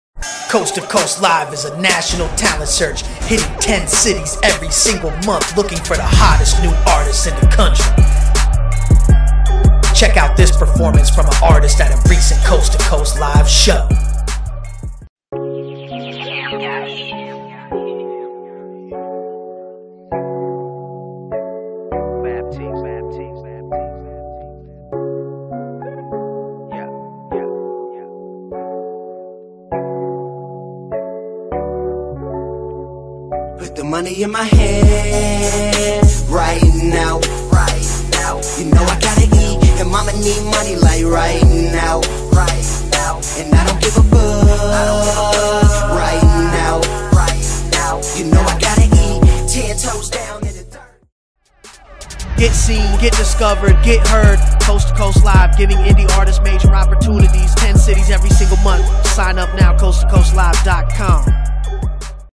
Baptiste Performs at Coast 2 Coast LIVE | Kansas Edition 3/22/16 - 2nd Place
Artist
Baptiste - My passion for what I do is indescribable. I know this is what I am meant for and I''''m coming to get it. This isn''''t a game to me i want it to...

DJ Macc - Official Core DJ
DJ Swayd - On Air DJ for KZUM 89.3 FM Lincoln, NE and 103.7 FM Da Beat Rockford, IL
Epademik Productions - 1/3 of The Deadbeatz - Producers for Rick Ross, French Montana, Joell Ortiz

Tuesday, March 22, 2016 9:00 PM

The Roxy
7230 W 75th Street